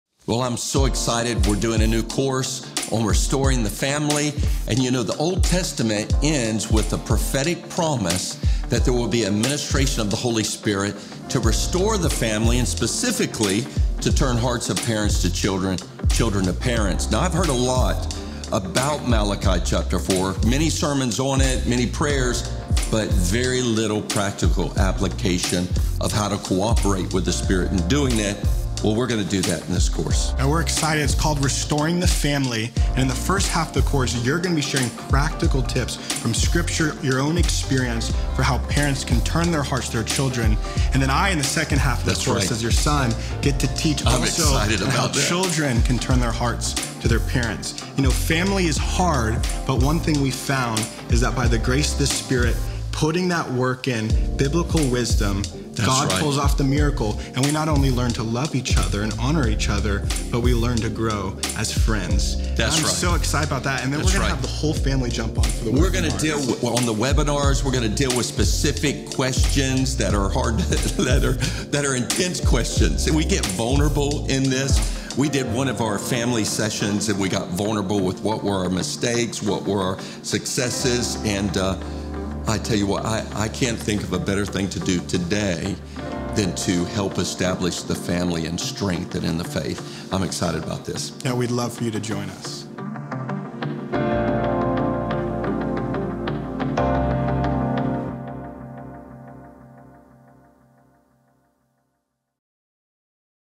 Restoring the Family | Official Trailer | Allen Hood Online
This Oct. 17 we are launching a new online course called “Restoring Family” geared towards equipping parents and children alike in navigating the complexities of family life—especially during the teenage and young adult years—and build deeper friendships with one another.

I am so excited to announce that I will be co-teaching this course on family with oldest son Samuel. In the first half of the course, I will teach practical ways for parents to turn their hearts to their children. In the second half of the course, Samuel will teach practical ways children can turn their hearts to their parents.

If you are interested in this course, you can learn more and sign up at allenhoodonline.com. The last day to register is October 16. We’ve love to have you join us!